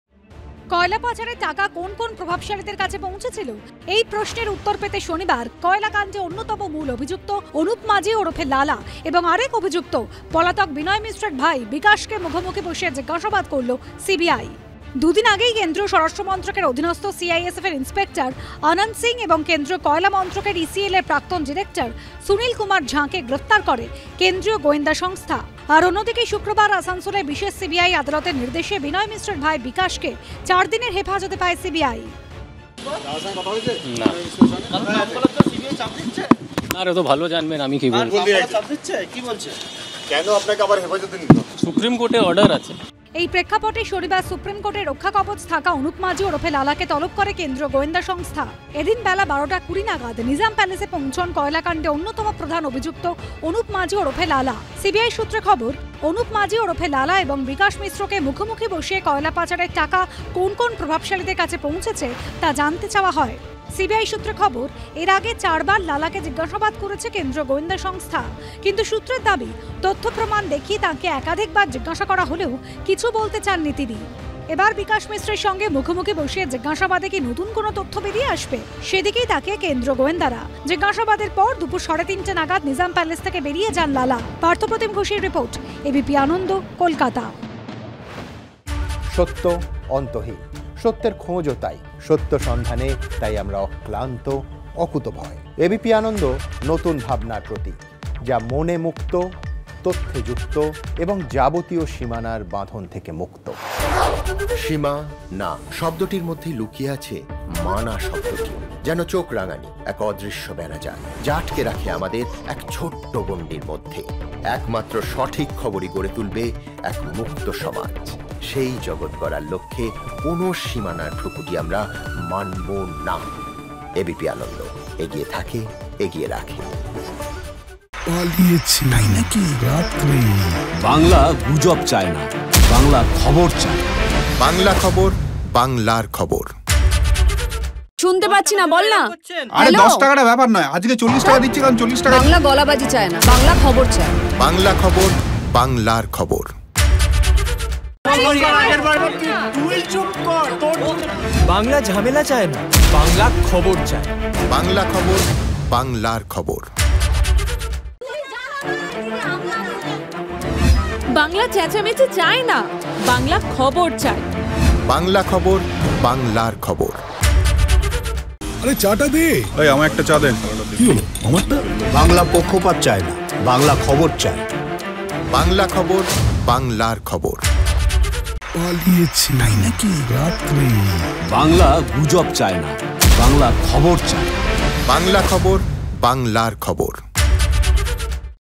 Coal Smuggling: কোথায় কোথায় কয়লা পাচারের টাকা? অনুপ মাঝি-বিকাশ মিশ্রকে জিজ্ঞাসাবাদ সিবিআইয়ের
কোথায় কোথায় কয়লা পাচারের টাকা? প্রভাবশালীদের লিঙ্ক খুঁজতে   ৩ ঘণ্টার বেশি মুখোমুখি জিজ্ঞাসাবাদ সিবিআইয়ের।